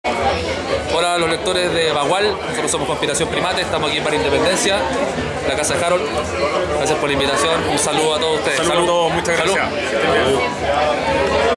Conspiración Primate en Bar Independencia
Banda Conspiración Primate manda un saludo a los lectores de Bagua,l desde Bar Independecia. Muy buenos muchachos en la música.